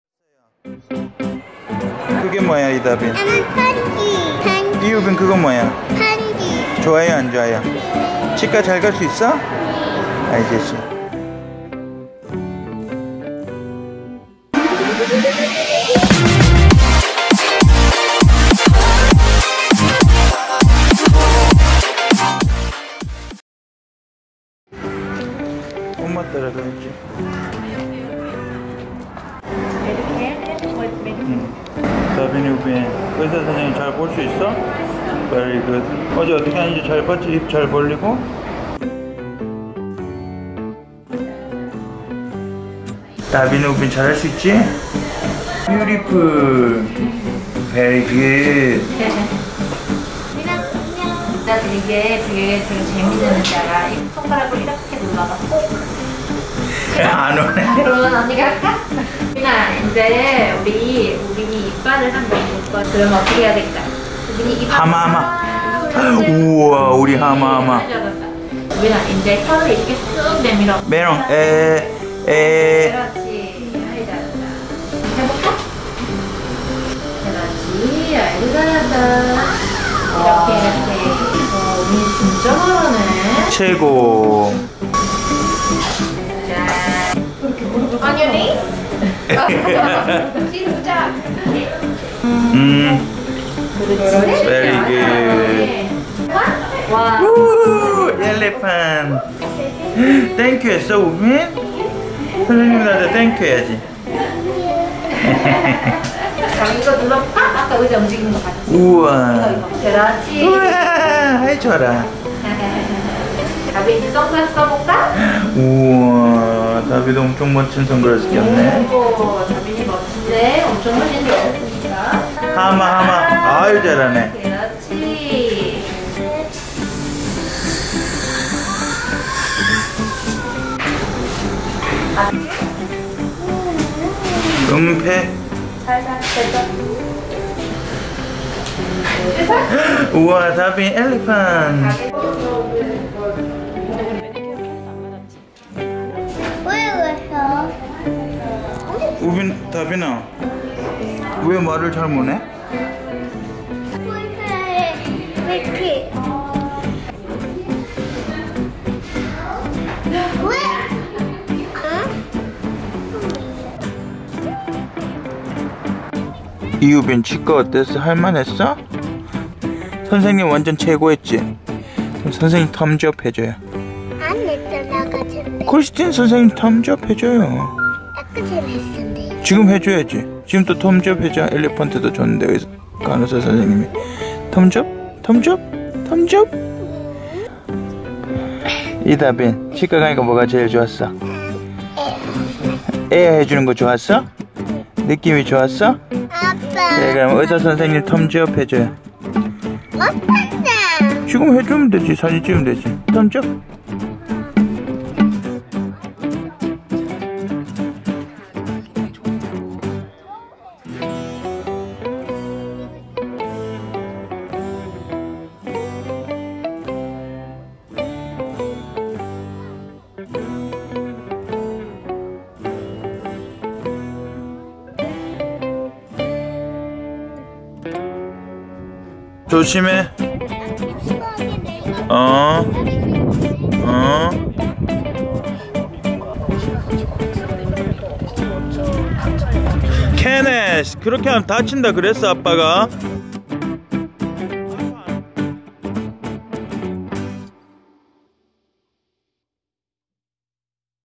[Suby's 호주생활] 호주 유아 무료 치과 진료, 우리집 2호 3호 치과 정기점검 받는날
호주생활 이야기 - 치과 진료 및 치료
호주 유아 치과 치료는 무료?
Child Dental Benefits Schedule
의외로 치과 치료 잘받는 2호 3호
어디서든 만나면 반가운 한국분!
치과의사 선생님 쵝오